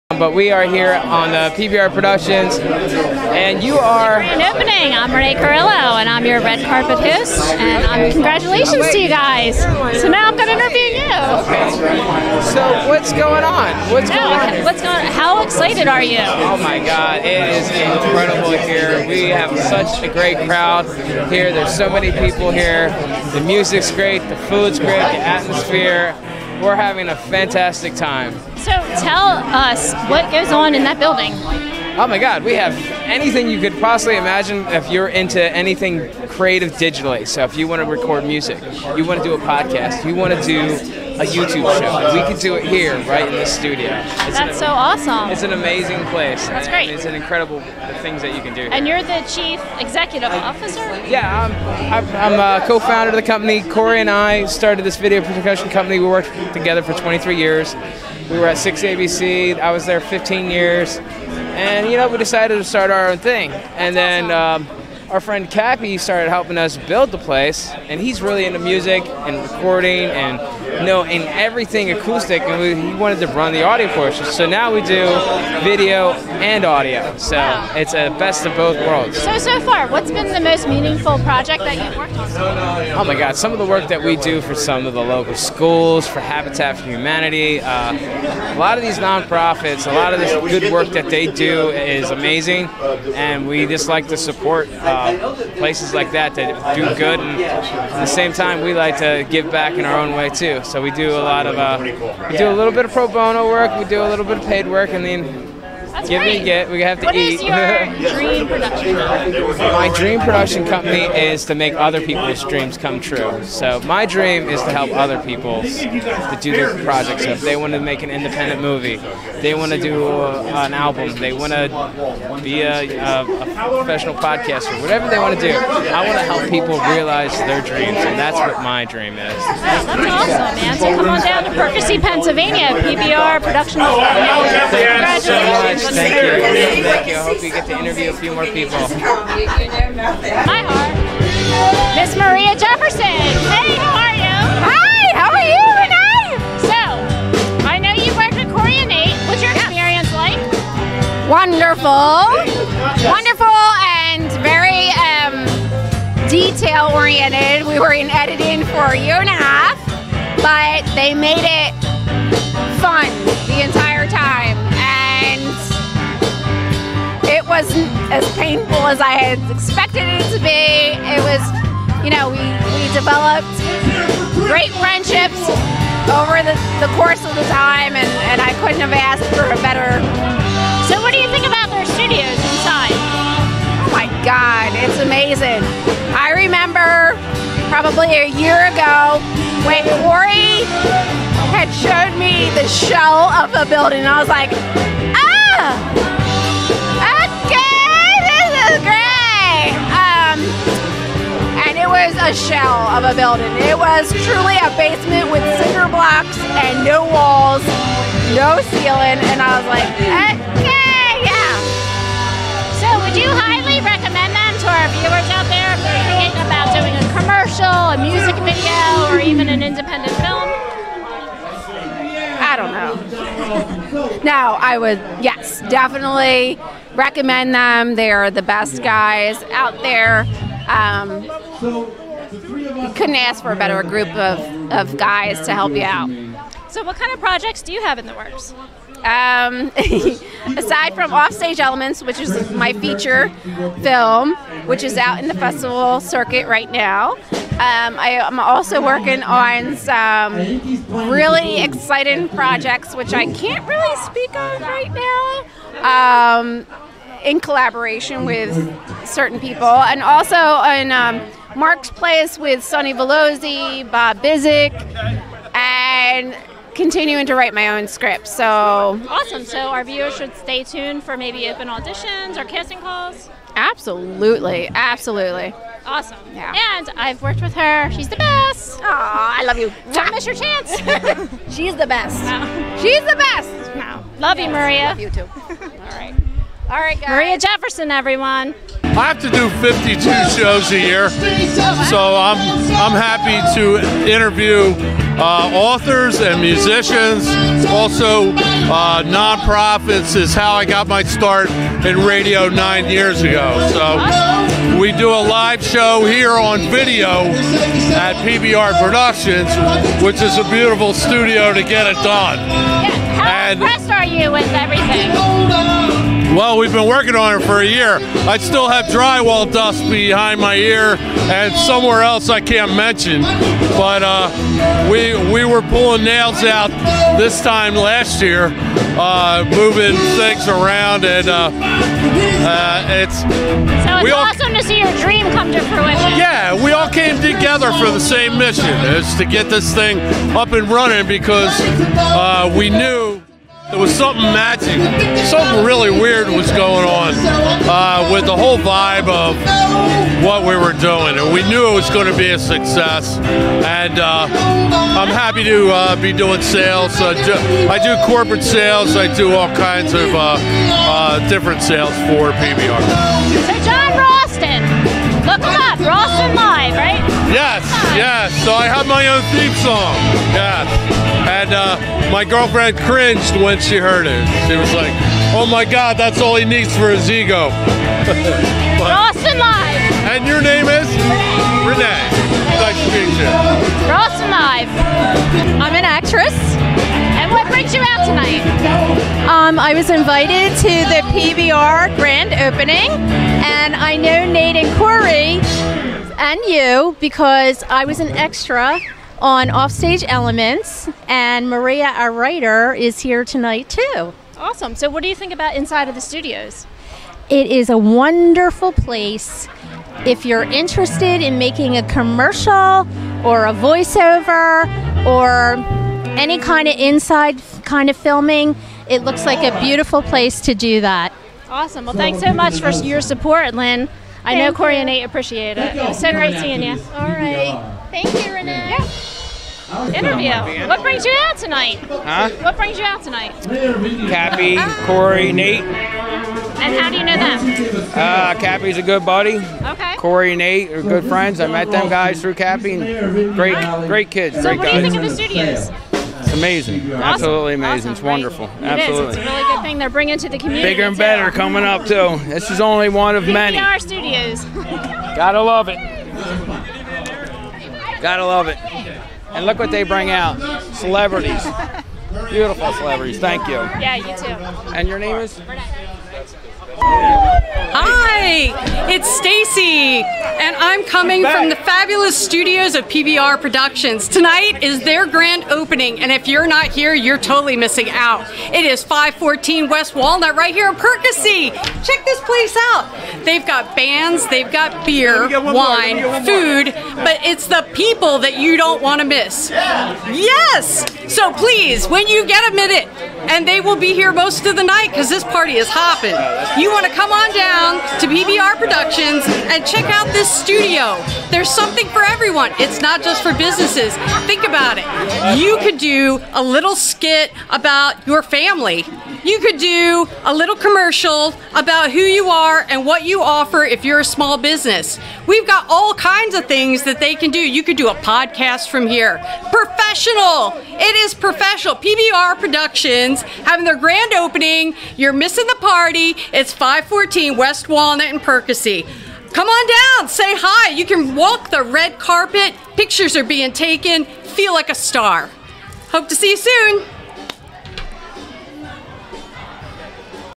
Red Carpet Interviews
Did you miss the big party for our studio grand opening? Check out some of the Red Carpet interviews with the lovely Renee Carrillo.  More to come soon.